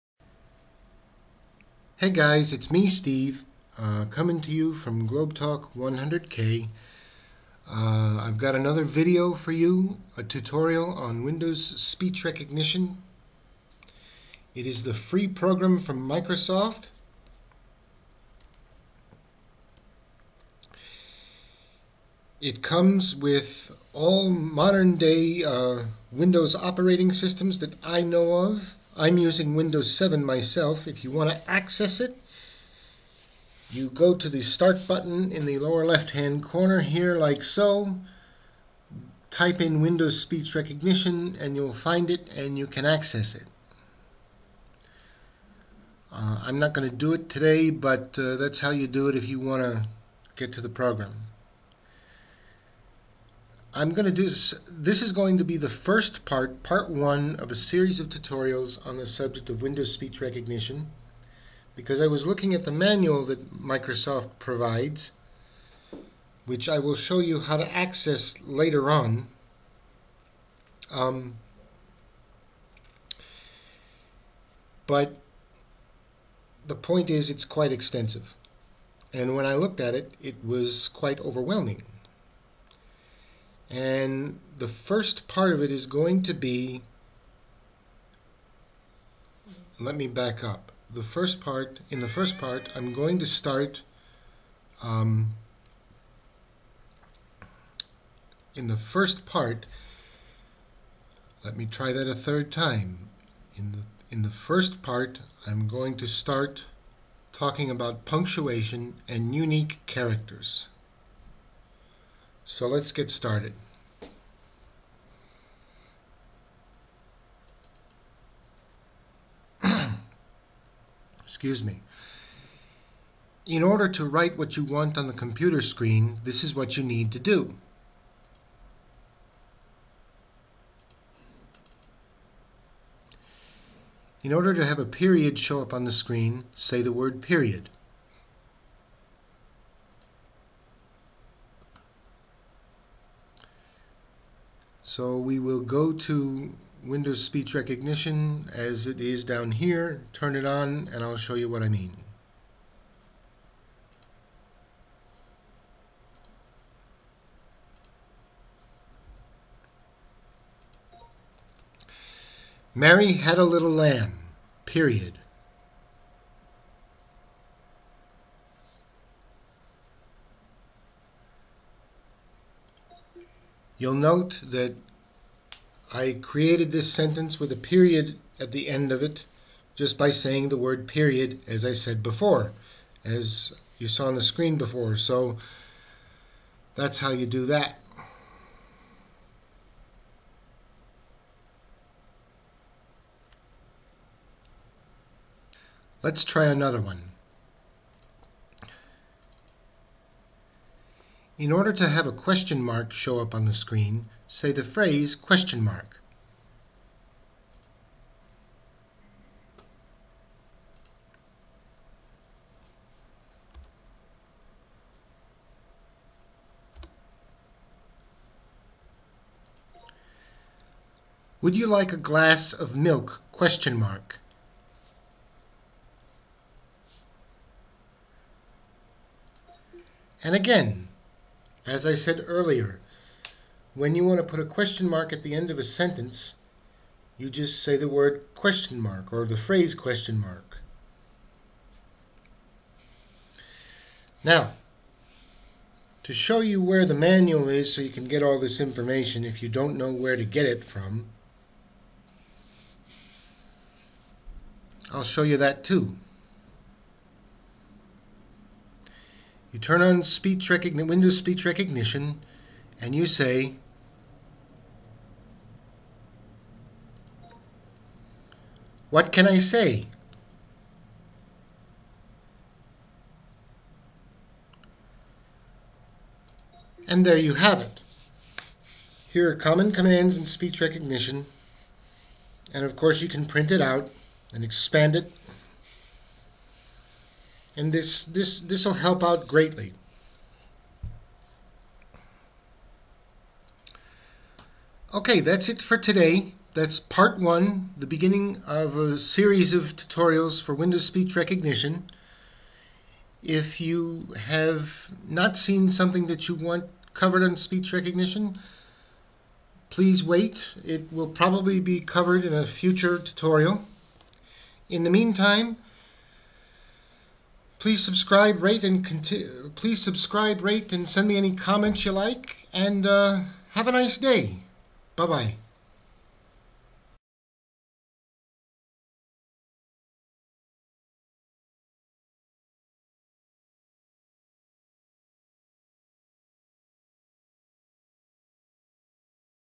Windows Speech Recognition: Part 1 - Basic Punctuation & Other Characters
This is part 1 in my series that covers Windows Speech Recognition. Obviously, there will be additional uploads on this subject in the near future. Enjoy!